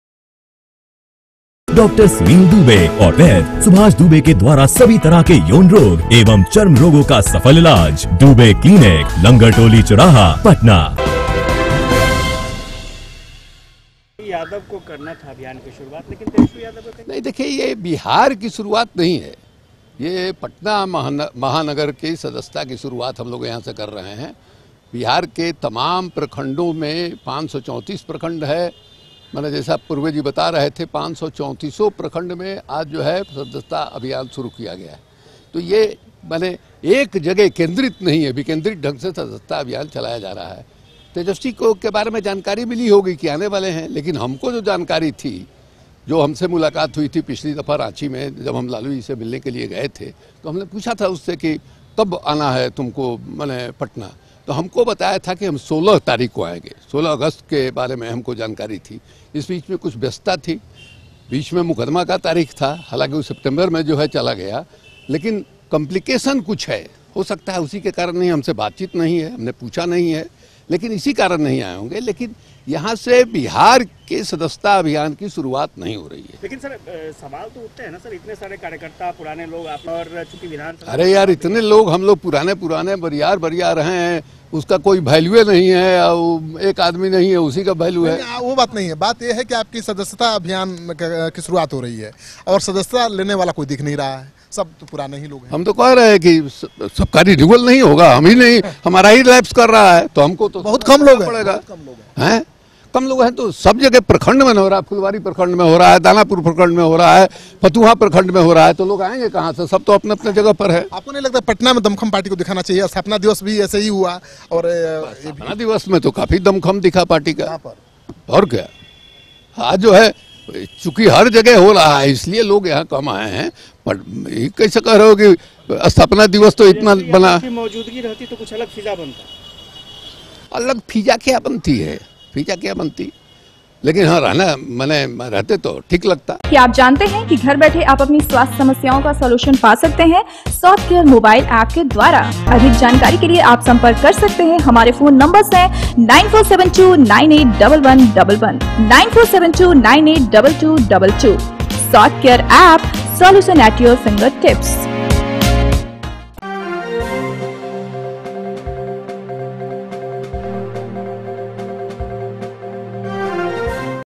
तेजस्वी यादव ने नहीं मानी सलाह, जवाब देने में लटपटाने लगे हैं शिवानंद तिवारी
City Post Live Special - तेजस्वी यादव ने नहीं मानी सलाह, जवाब देने में लटपटाने लगे हैं शिवानंद तिवारी |